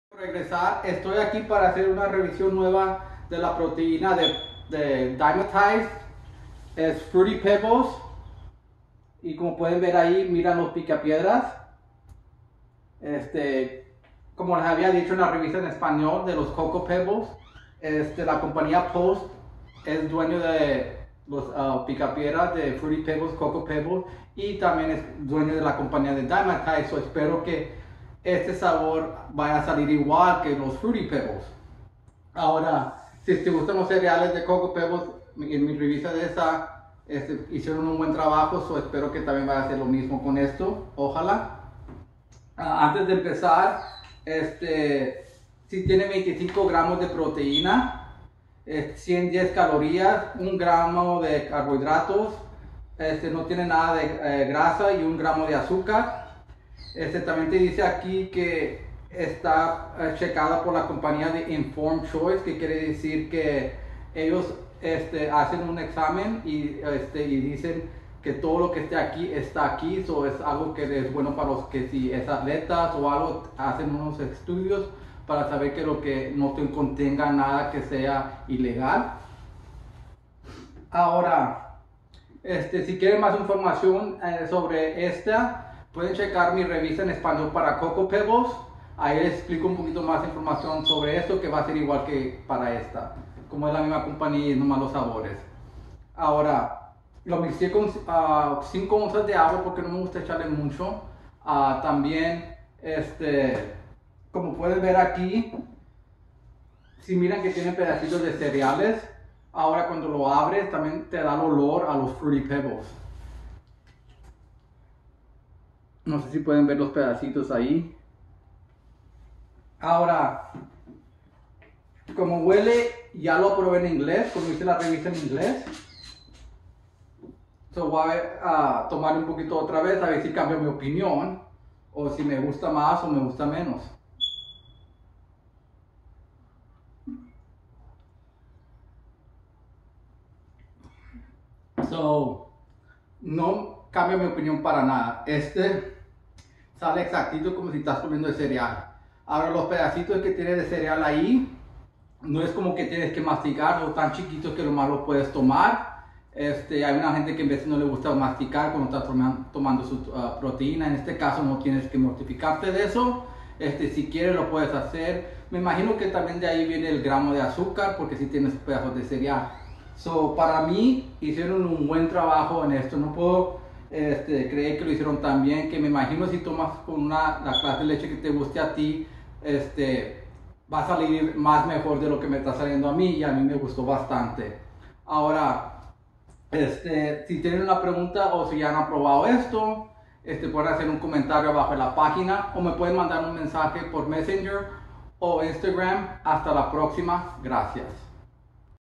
Dymatize Proteína Iso 100 Revisa Picapiedras Fruity Pebbles
Dato breve: Publique los propios Fruity n Cocoa Pebbles junto con Dymatize.

Calorías 110
Proteína 25gm
Carboidratos 1 gm
Azúcar 1 gm
Certificado por Informed CHOICE org.

Revisión

Estaba realmente emocionado de probar este sabor porque realmente disfruté los guijarros de Cocao, que es uno de mis cereales favoritos. Como siempre hago en mis revisiones, mezclé un poco menos de 6 oz de agua, ya que normalmente usaba entre 4 y 6 oz. Siendo que es un aislado de suero, es una proteína fina, por lo que se mezcla fácilmente. Cuando abres el recipiente, hueles a guijarros afrutados. También puedes ver pequeños trozos de cereal. Tan pronto como lo probé no podía negarlo. Sabía a guijarros afrutados y esto era con agua. Guau. Es como la leche sobrante del cereal. Hicieron un trabajo fenomenal que tiene sentido ya que son dueños de ambas compañías, lo que significa que conocen la fórmula de su cereal. Entonces sí, sabe a guijarros afrutados. No hay necesidad de explicar o elaborar mejor. Si eres un fanático de los guijarros afrutados, entonces obtienes guijarros afrutados y no bucles de frutas o trix, etc.